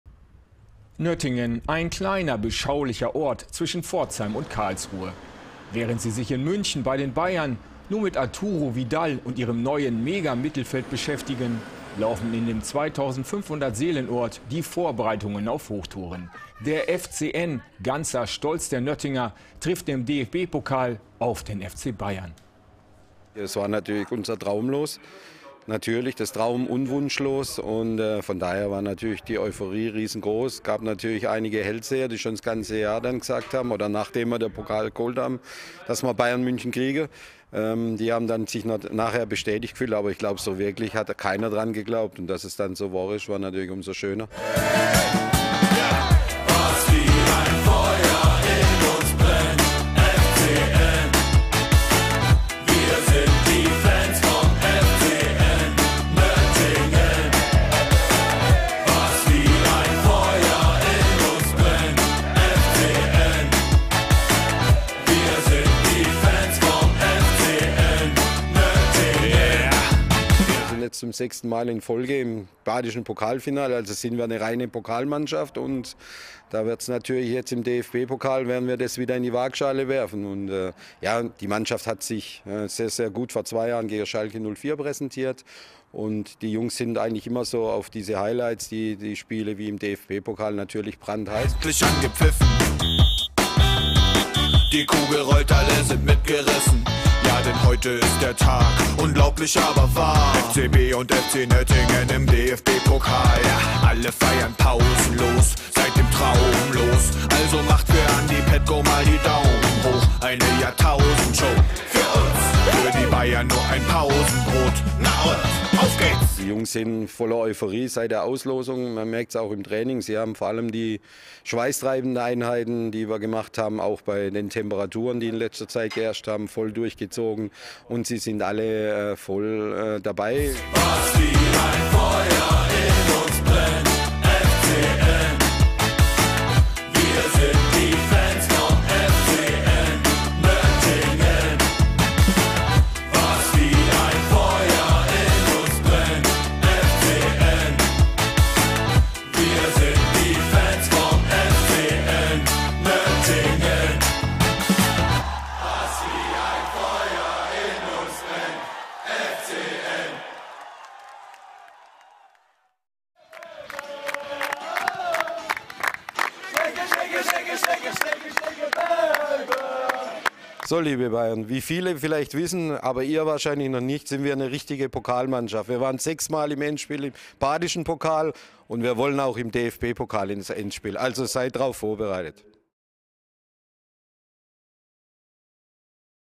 "Was wie ein Feuer brennt" - Nöttingen ist heiß auf Bayern - kicker.tv
Im beschaulichen Nöttingen ist alles eine Nummer kleiner als in der Großstadt München. Trotzdem oder gerade deswegen freut man sich in Baden riesig auf den deutschen Rekordmeister. Die Euphorie vor dem DFB-Pokalspiel gegen die Bayern ist groß und diese wurde nun in Form eines Videos vom FC Nöttingen umgesetzt. Trainer Michael Wittwer warnt das Münchner Starensemble: "Wir sind eine Pokalmannschaft."